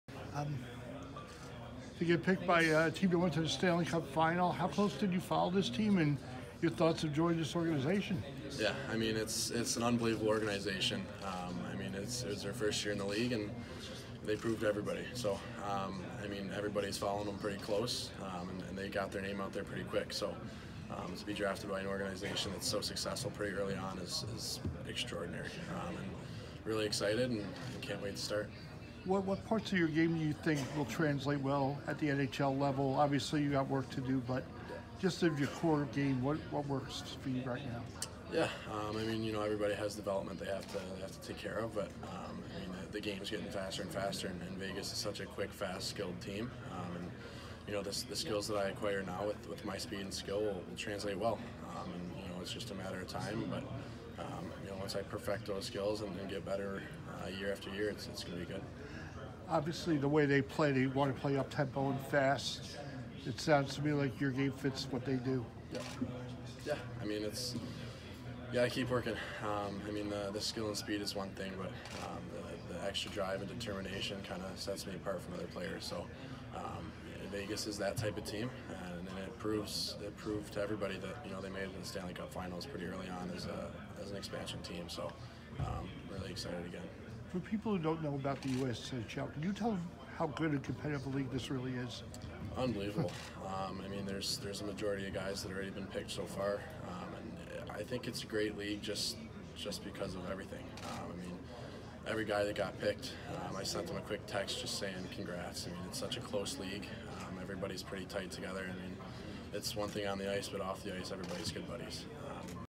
Paul Cotter | 2018 NHL Draft
Canton native Paul Cotter talks about being selected by the Las Vegas Golden Knights in the 2018 NHL Draft.